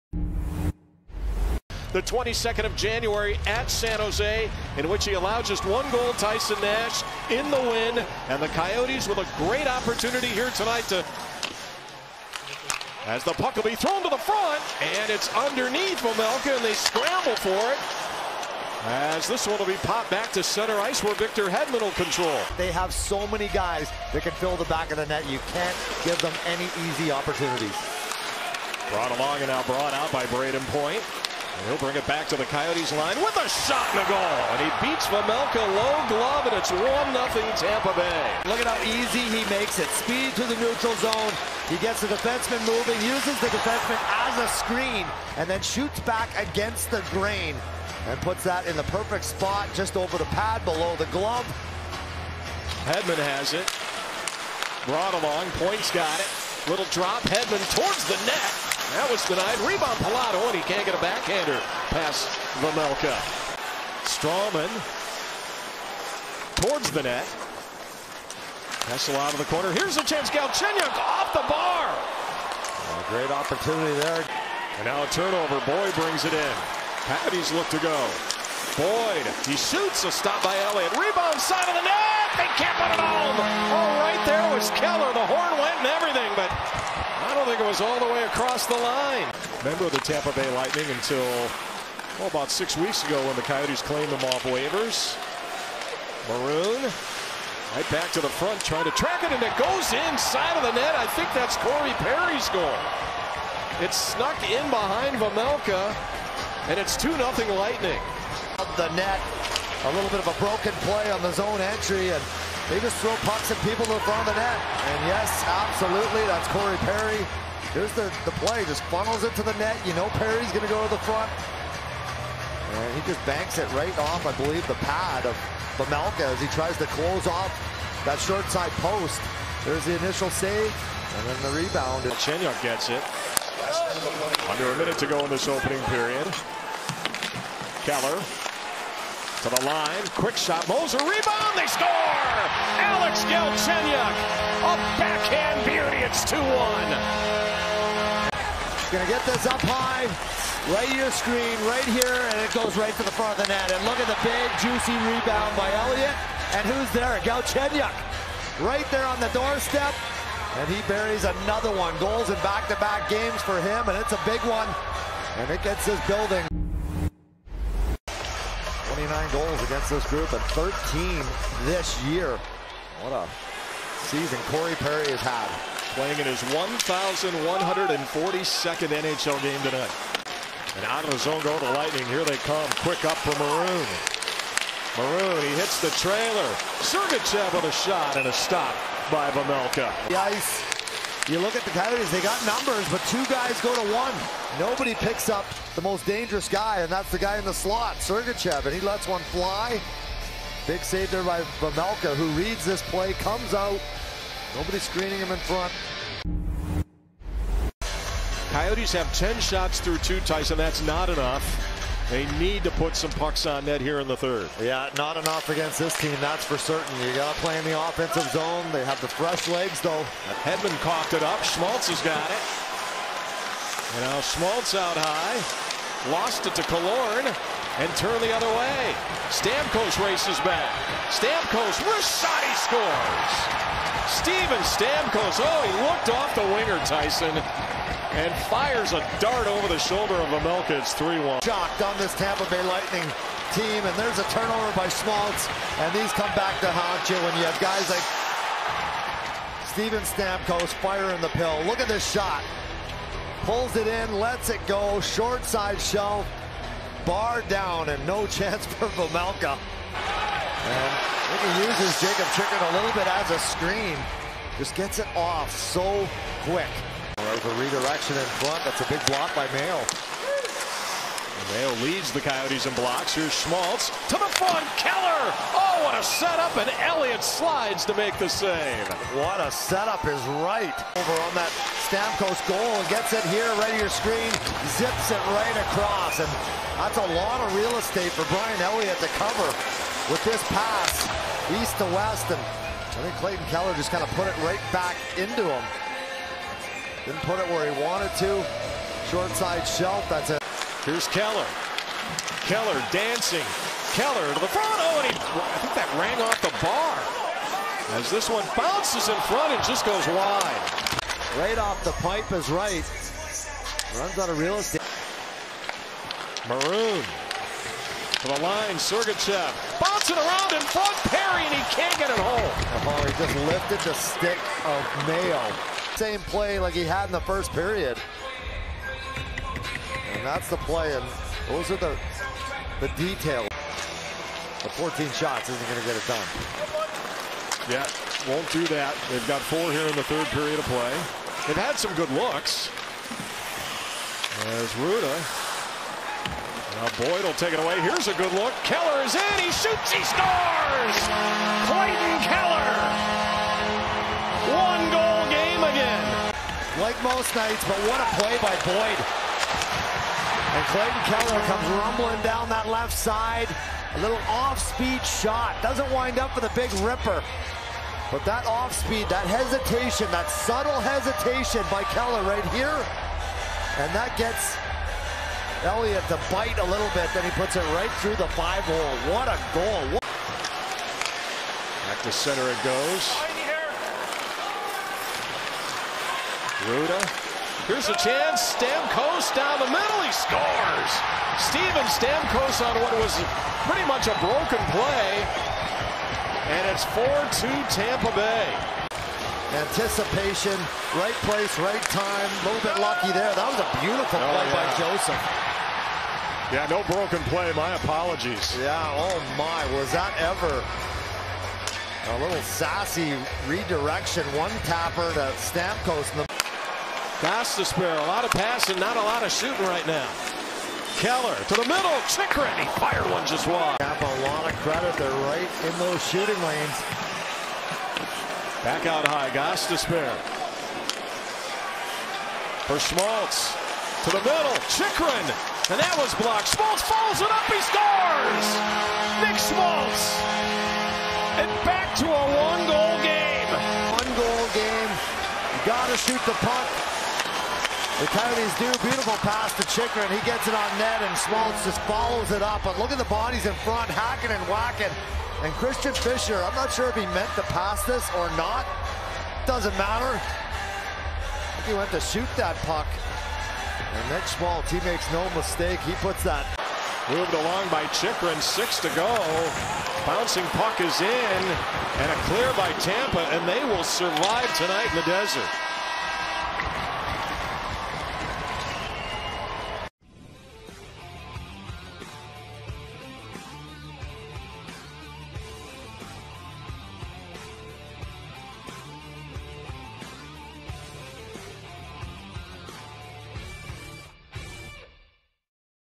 NHL Highlights | Lightning vs. Coyotes - Feb. 11, 2022
Steven Stamkos notched two goals including the game-winner as the Tampa Bay Lightning defeated the Arizona Coyotes 4-3.